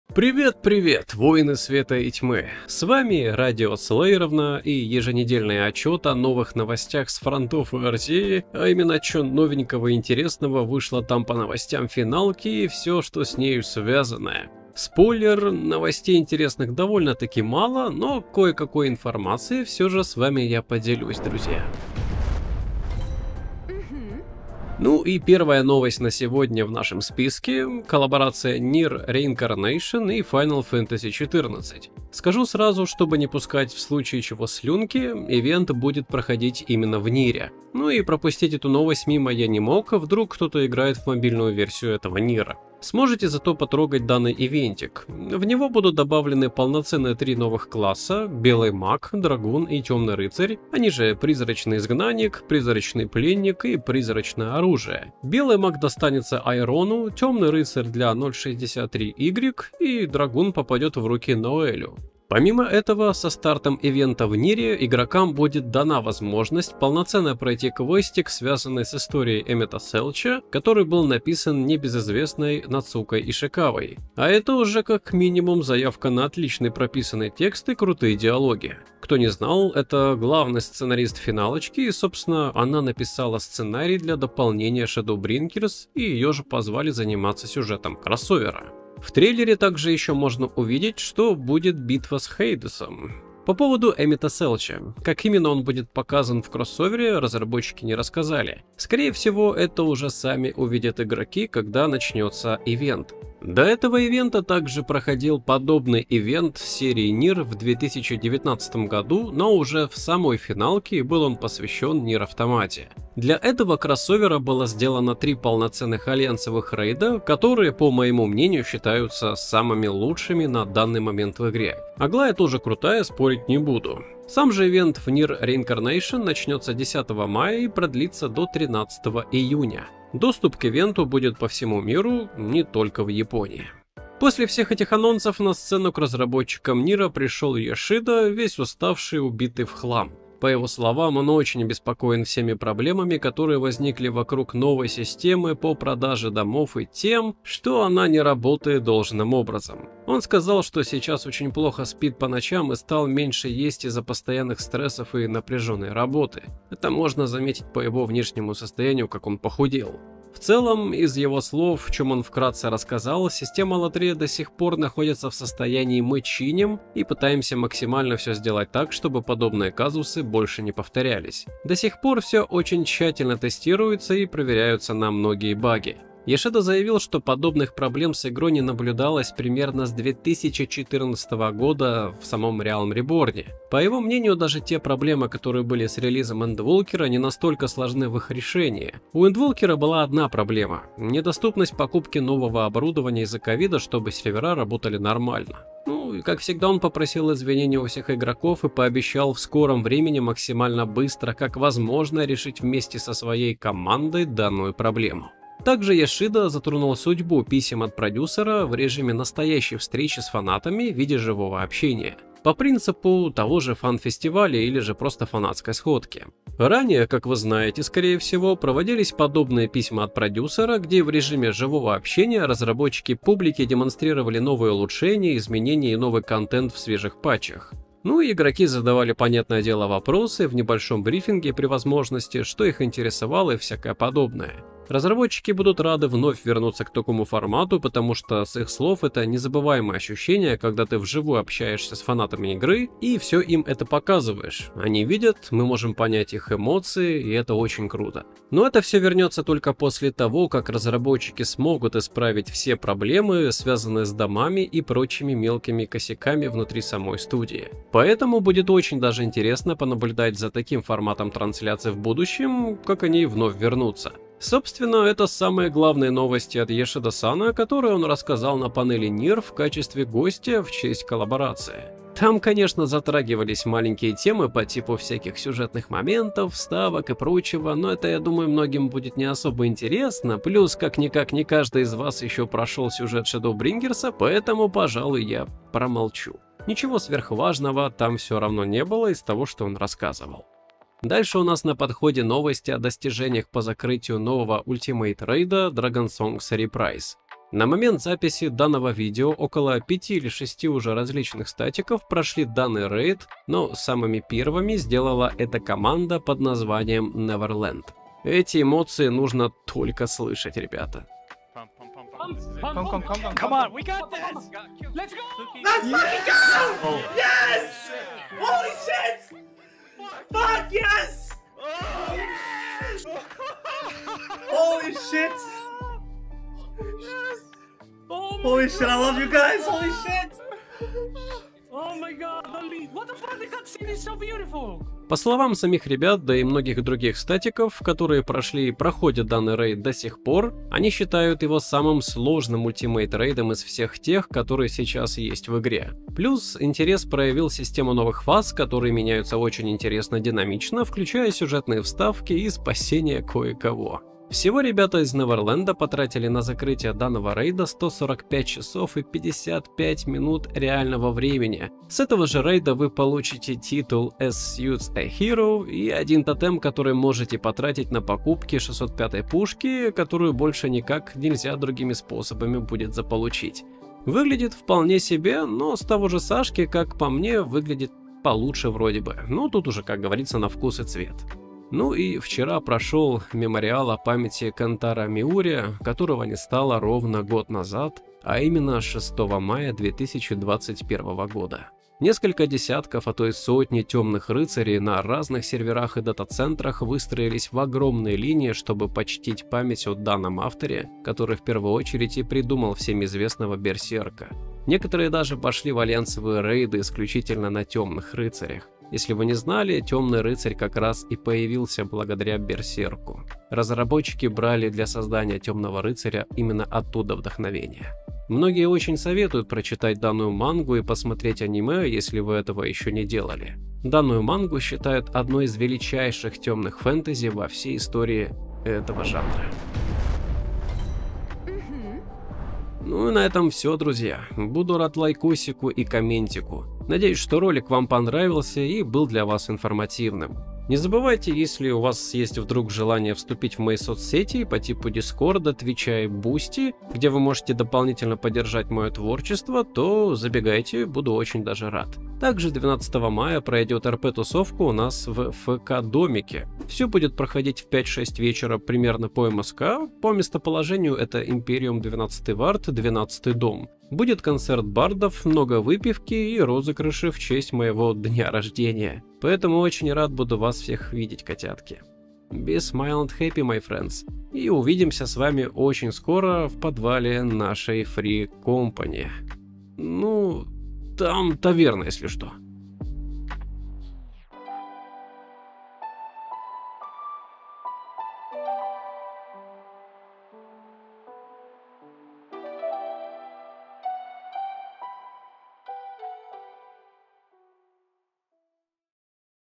(Новости FFXIV) Коллаб с мобильным Ниром, закрытие нового ультимейта и немного про "лотерею" домов
Пачка новеньких еженедельных новостей по финалочке. Тут и про новый коллаб FFXIV c NieR: Reincarnation (для второй), немного новостей про "лотерею" с домами, возвращение LFP в формате сходок с фанатами, первое мировое закрытие нового рейда "Dragonsong's Reprise" и дань уважения о памяти великого Кэнтаро Миуры, создателя "Берсерка".

Be smile and happy my friend ^^

Таймкоды:

00:00 Вступление
00:23 Коллаборация NieR: Reincarnation и Final Fantasy XIV
02:14 Немного информации о состоянии “лотереи” с домами
03:25 Письма от продюсера будут вновь проводится в реале
04:51 Первый мировой рекорд по закрытию нового ультимейта “Dragonsong’s Reprise”
06:21 В память о Кэнтаро Миуре...
07:11 Заключение